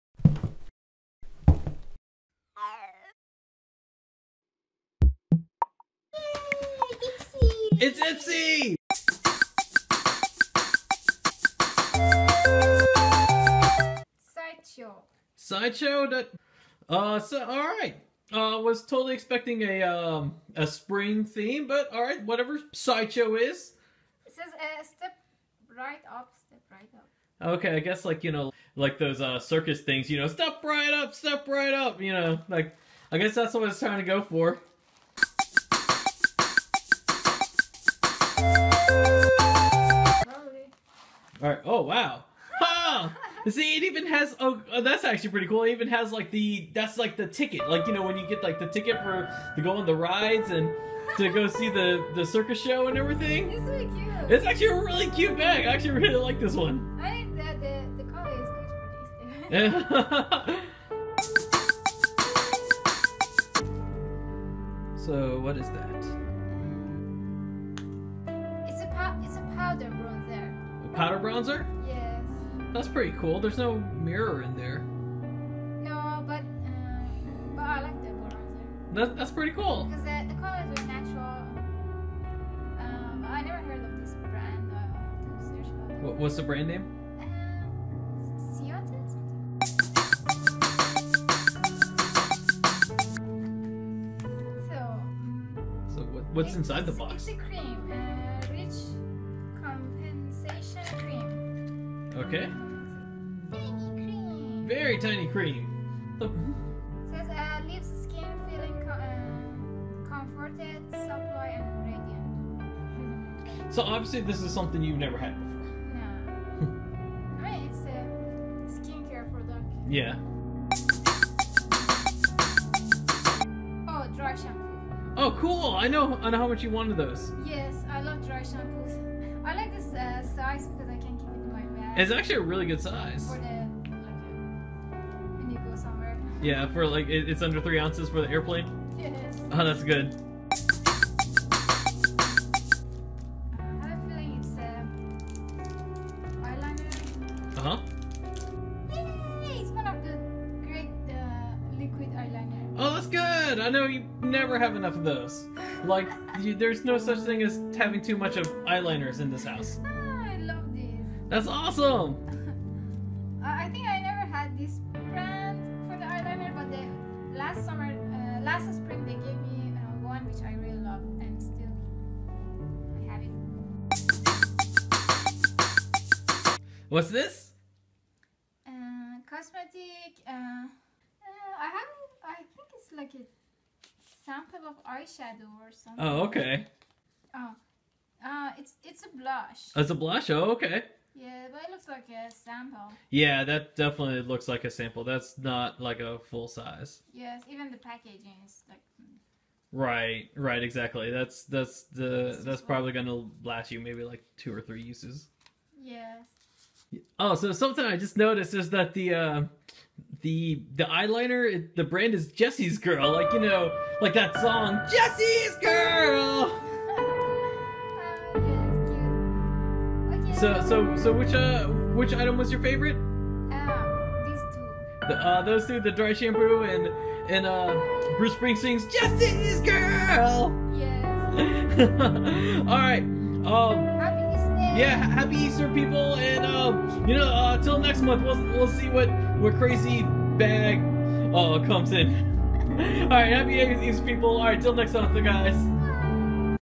IPSY Make Up Bag Unboxing/Unbagging - April 2017 Side Show
Ipsy make up glam bag unboxing video.Month of April and theme is "Side show",unboxing by Moving Lakposht Studio
Step right up-Step right up
its spring and we love to apply that black eyeliner and red red lipsticks again. Ipsy is very wellknown for its creative design make up bags and natural beauty products. this month the bag was very creative,the theme was " side show" and they shared the stories of two of their models which they have different style but they are proud to be exactly what they are. in this unboxing I also present what I am exactly and I love to have my own creative style for beauty unboxing. I add all necessary informations from the sources and you will find so many fun and honesty in our unboxings,hopefully you enjoy it and support my video by like and comment.

BE SOCIAL!!